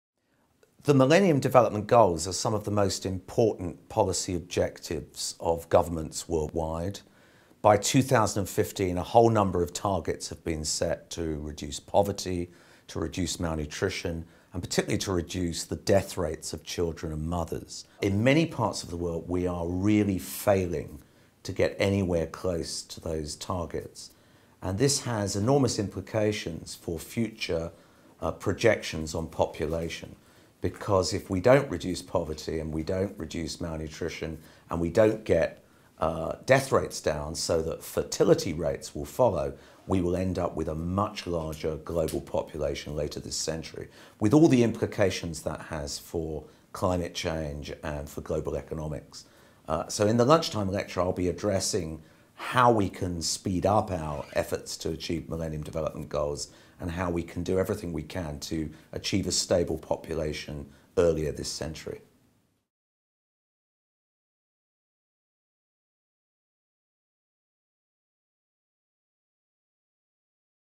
UCL Lunch Hour Lecture taster: Prof Anthony Costello
A one minute taster for Professor Anthony Costello's Lunch Hour Lecture entitled "Stabilising the global population: Where next for the Millennium Development Goals for health and nutrition?" Taking place at UCL on 25 January 2011, 1.15PM

SUMMARY: In many poor countries the Millennium Development Goals for improvements in nutrition and health, especially of mothers and children, will not be met by the target date of 2015.  This talk will review progress towards these targets and consider critical obstacles to success.  New strategies will be considered to improve nutrition and to accelerate reductions in death and fertility rates so that the global population will be stabilised by mid-century.